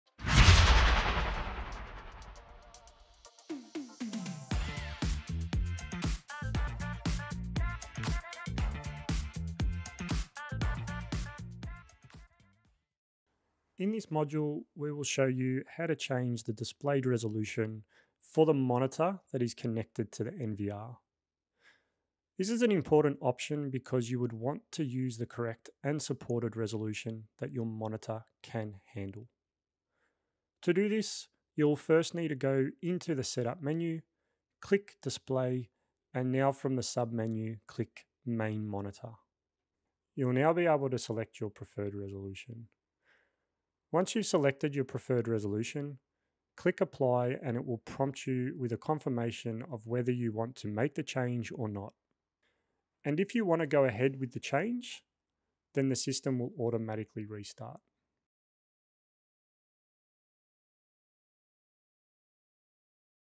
Change Display Resolution (Module 2) - IDIS Training
This is the Change Display Resolution module from our IDIS NVR Self Paced training course.
This course has been designed and developed by our DAS Professional Training Services team.

In addition, we offer facilitated training courses with discounted IDIS NVR kits that you get to walk away with.